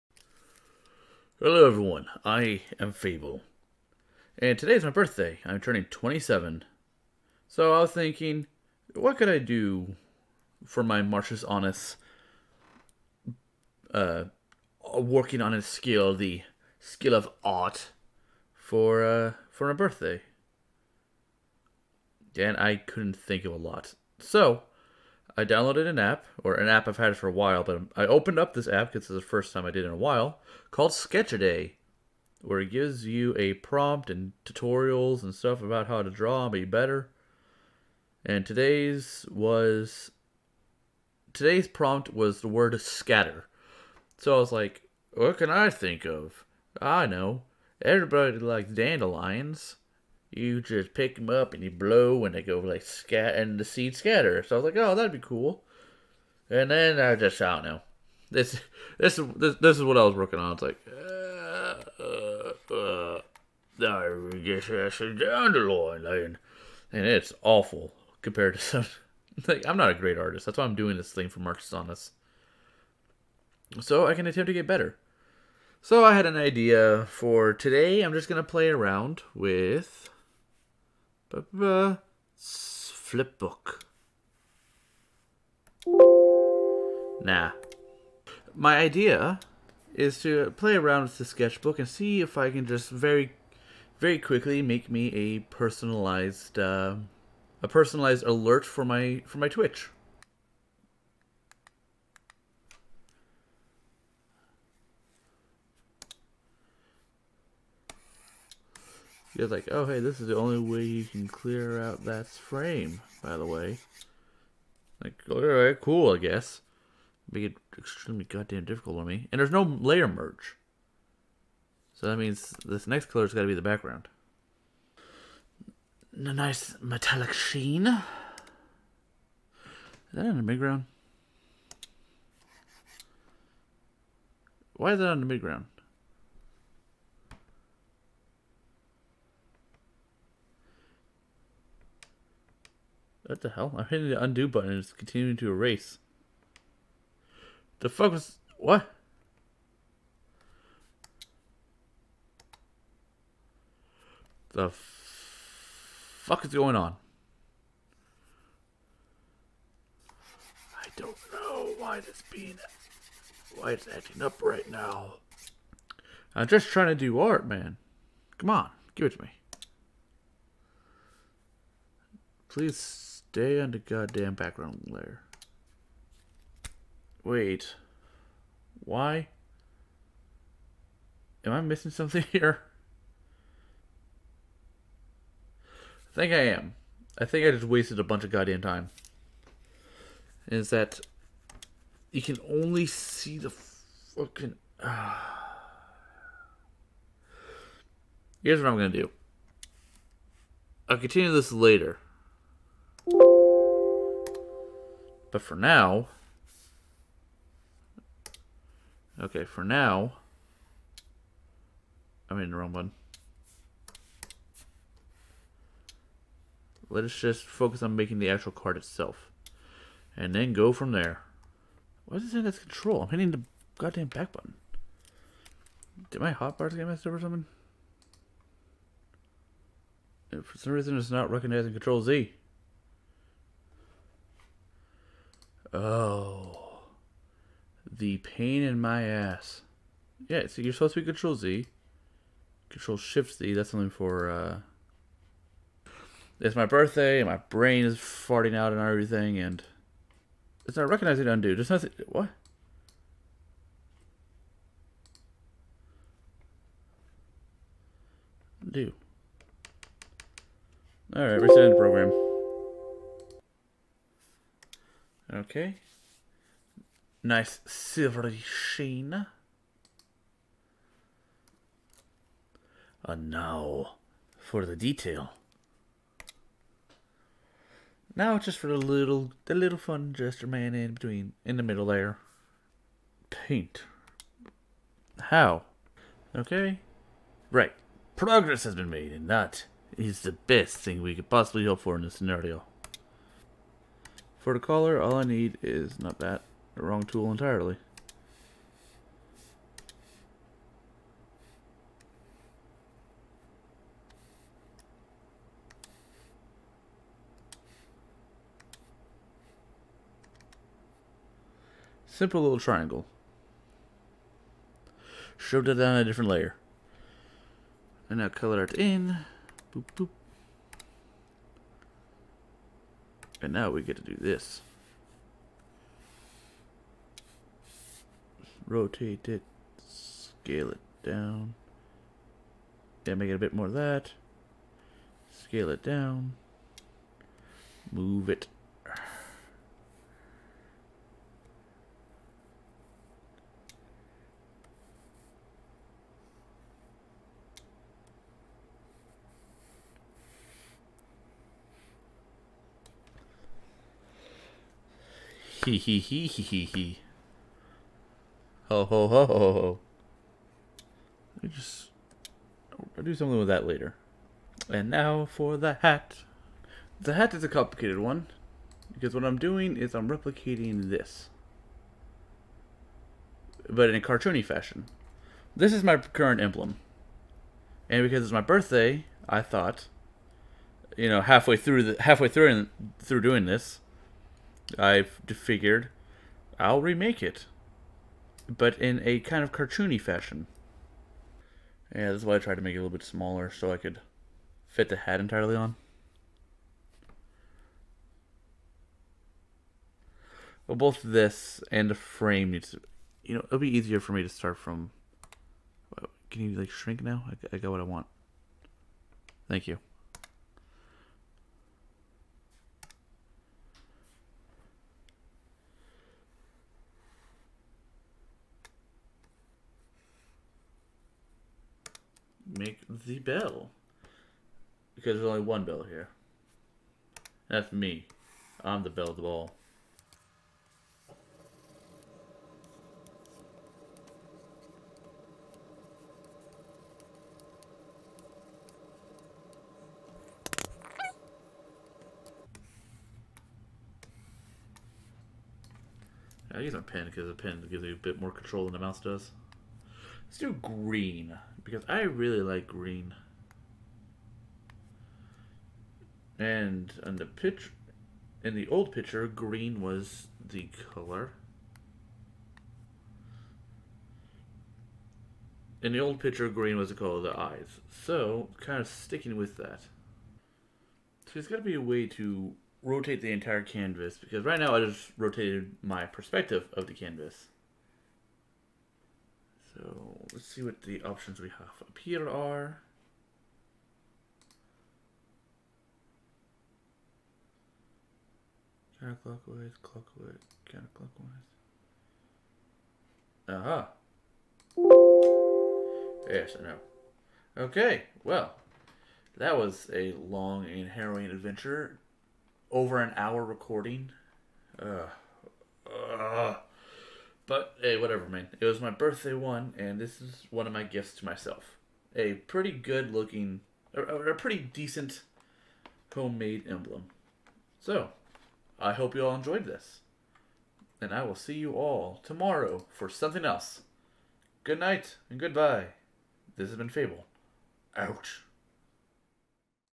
New Emblem! || MarchusAnnus || Day 6
For my birthday, I make myself a new emblem! Only regret was not making it round.